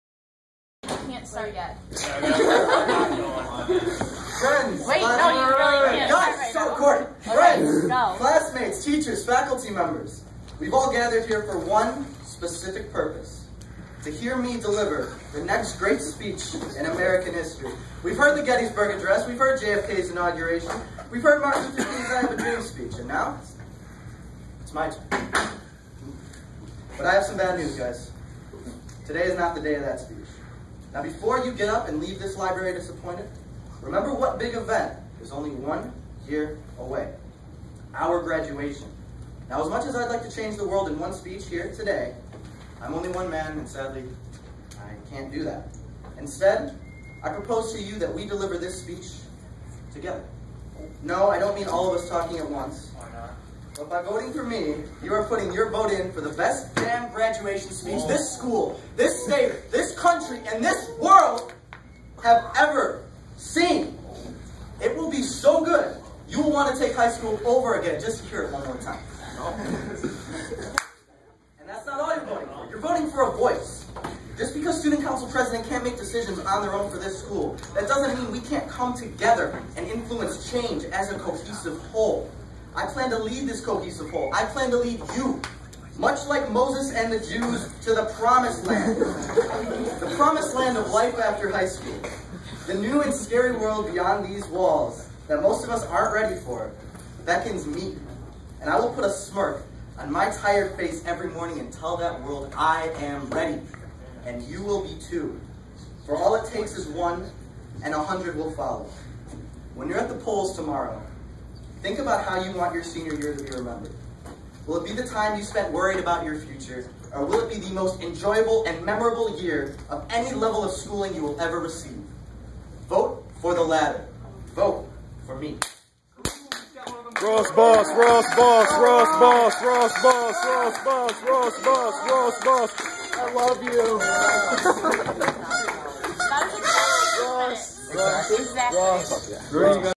Ross Pinsler gives the greatest Highschool presdental winning speech ever
Class pres 2013 Ross Pinsler gives the greatest Highschool presdental winning speech ever